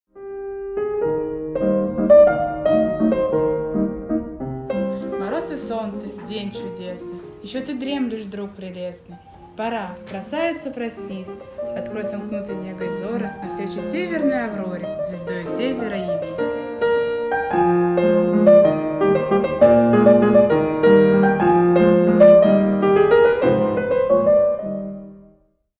Алевтина Щербина "Зимнее утро" А.С.Пушкин
Литературный смартмоб "Читаем Пушкина"
Воспитанница Отдела декоративно-прикладного творчества Дворца детского (юношеского) творчества Фрунзенского района Санкт-Петербурга
Алевтина Щербина
Стихотворение "Зимнее утро" А.С.Пушкин